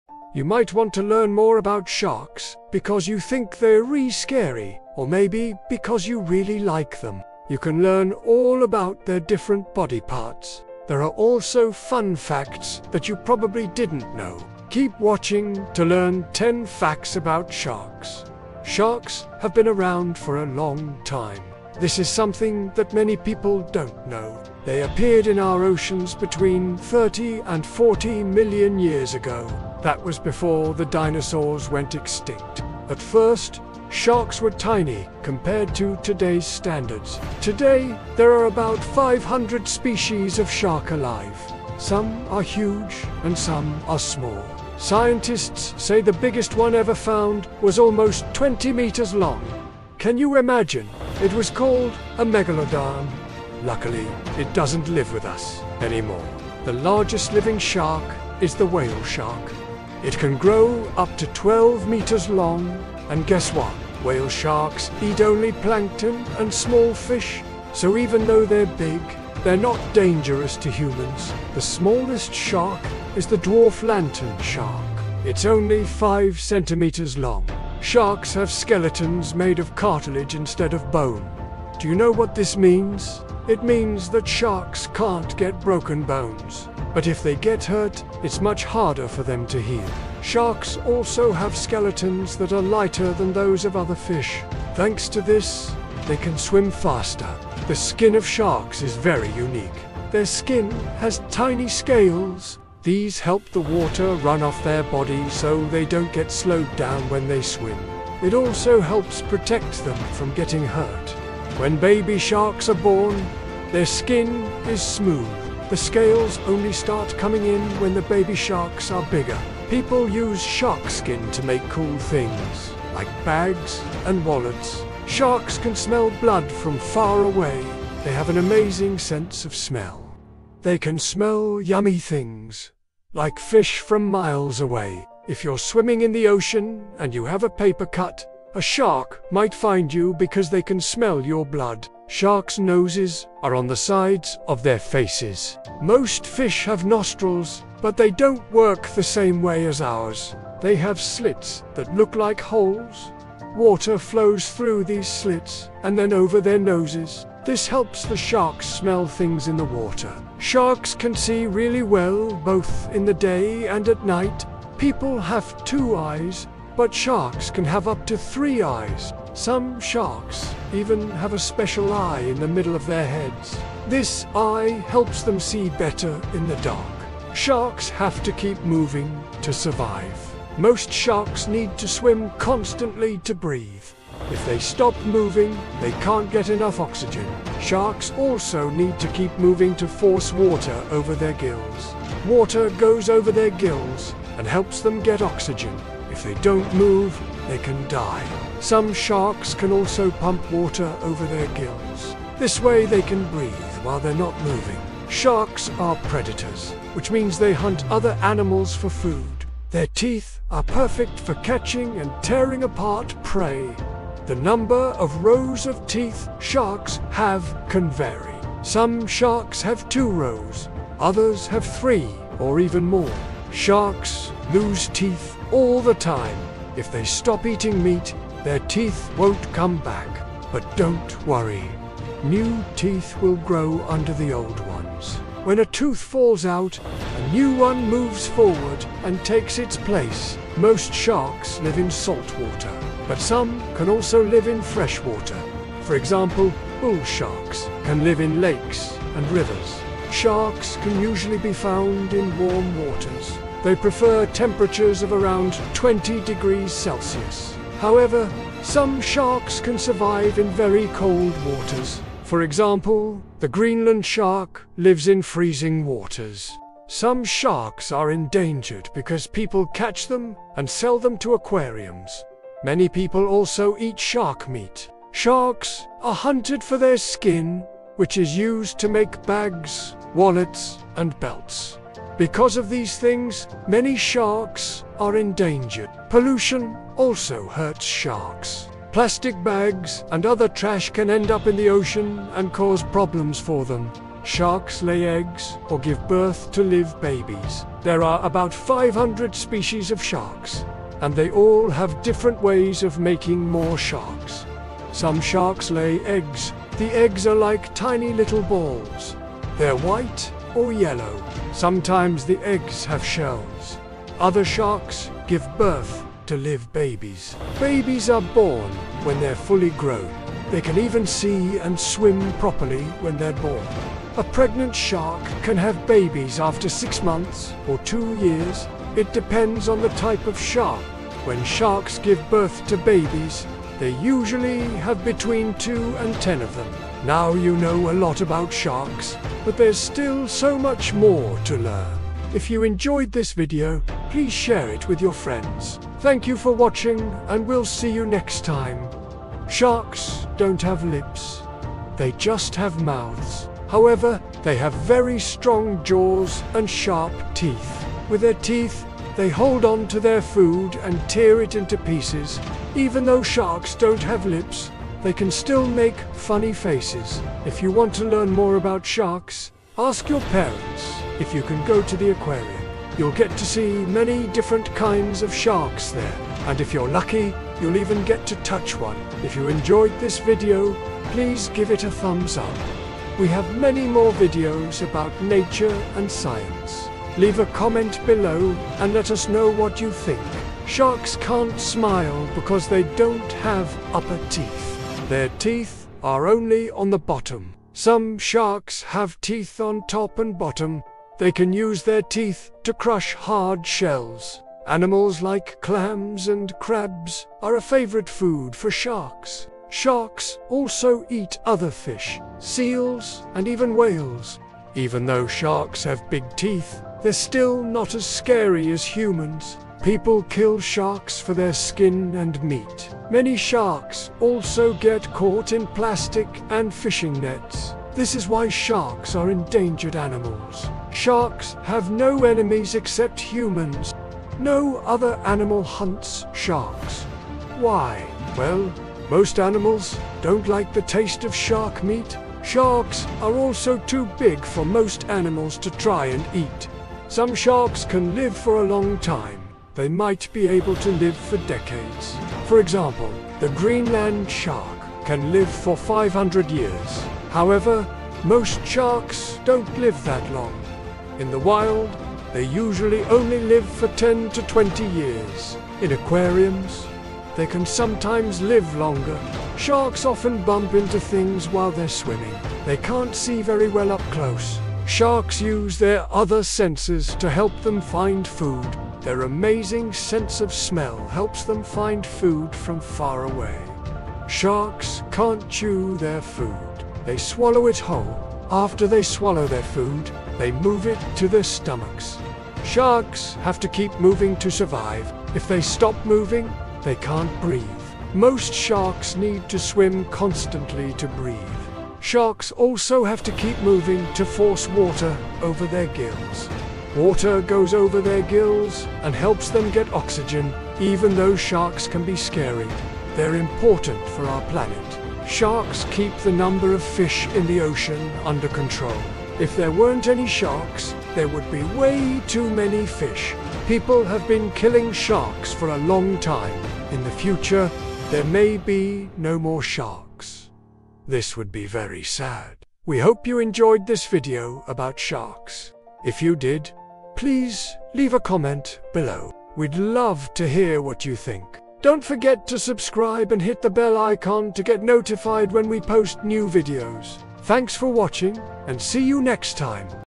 Shark Superstars! Dive into the Ocean's Coolest Fish! - Kiddios Universe
Ready for a deep-sea dive? 🦈🌊 Plunge into the ocean with us as we go Learning about Sharks! You'll meet incredible hammerheads, speedy mako sharks, the gigantic whale shark, and, of course, the mighty great white. Discover how sharks use their amazing senses, what they love to eat, and why they're so important to the ocean. This video is packed with thrilling facts and awesome underwater adventures that will make you a shark expert!